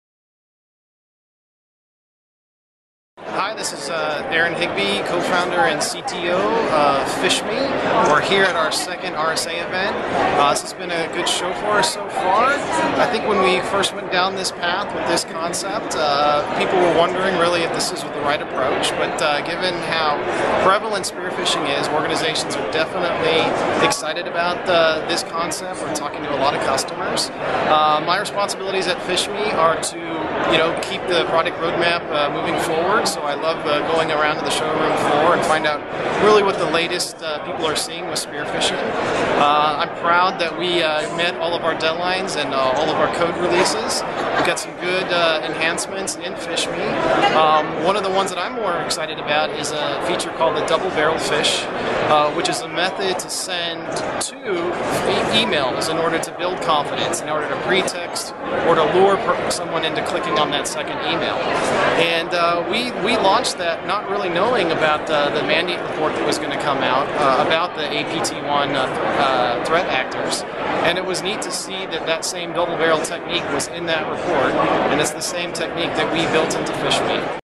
The Double-Barrel Phish
Today's IT Security Guru is Aaron Higbee, Co-Founder and CTO of PhishMe. Aaron discuss the 'Double-Barrel Pish'...

Find our friends: @Eskenzi